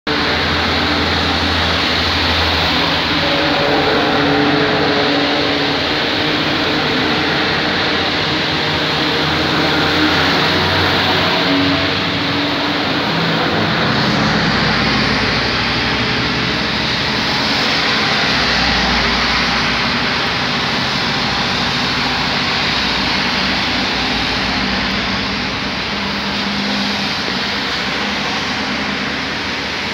Торопова дача. Севастополь. Голубые пионеры.
Пионеры Артека убегают от дождя.